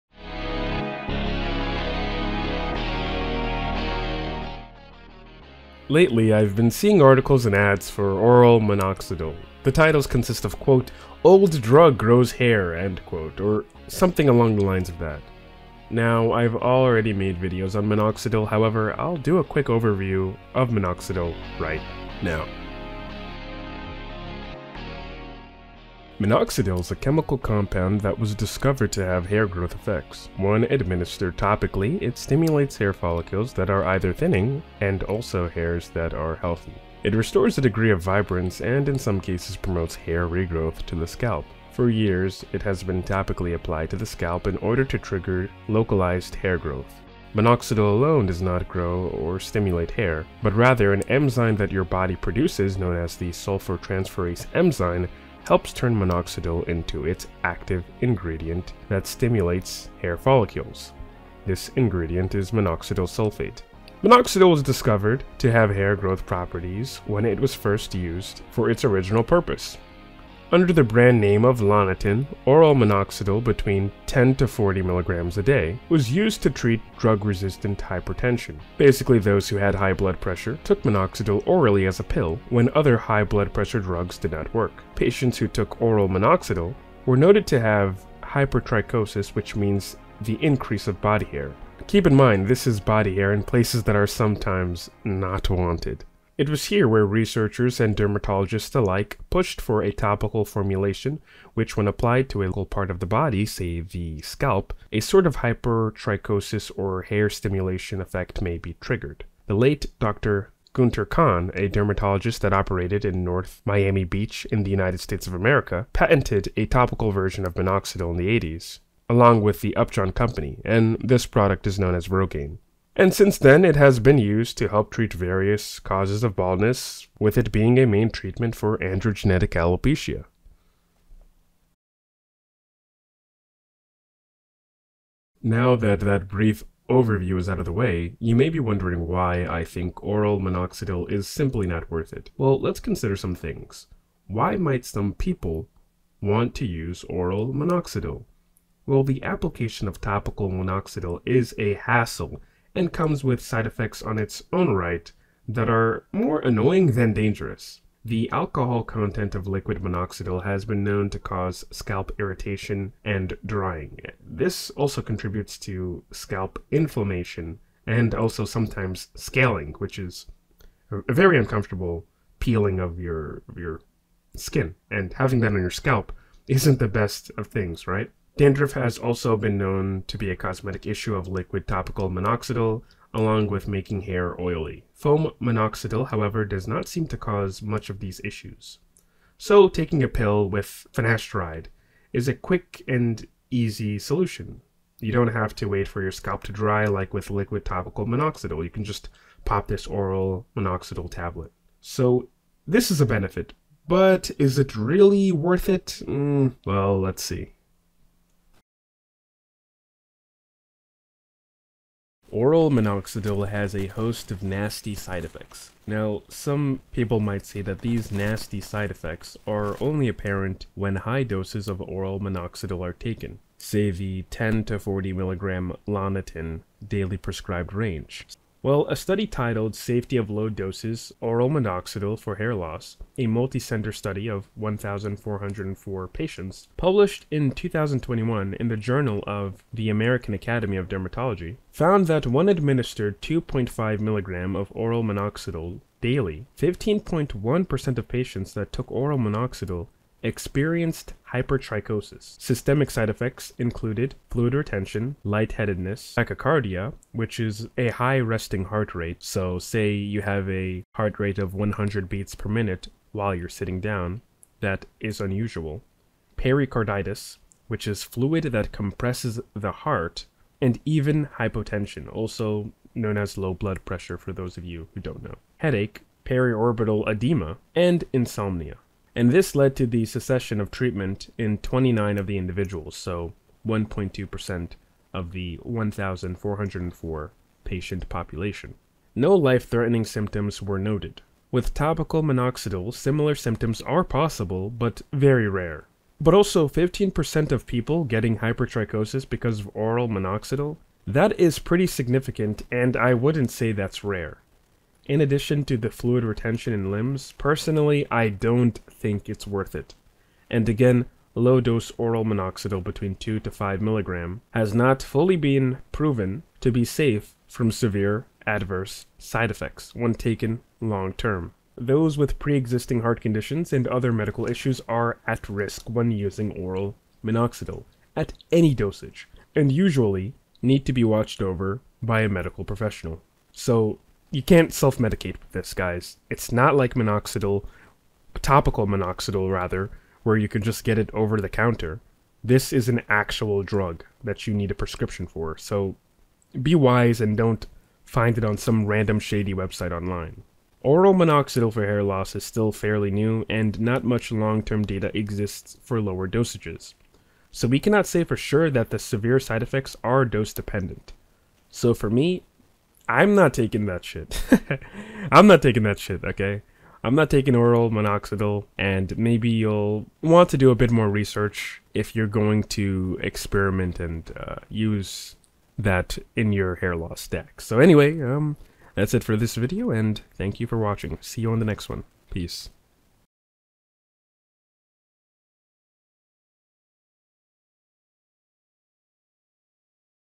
Oral Minoxidil: is it worth the risk?
Time Stamp:

0:23 What is Minoxidil?
2:11 Why Use Oral Minoxidil?
3:29 ***Downsides of Oral Minoxidil  (card naming error)

1. Minoxidil and its use in hair disorders: a review

2. Safety of low-dose oral minoxidil for hair loss: A multicenter study of 1404 patients

3. Clinical use of minoxidil (Loniten)

4.  Off-Label Use of Topical Minoxidil in Alopecia: A Review

4. Guinter Kahn, inventor of hair loss remedy Rogaine, dies at 80 in Miami Beach

5.  MINOXIDIL IN RESISTANT HYPERTENSION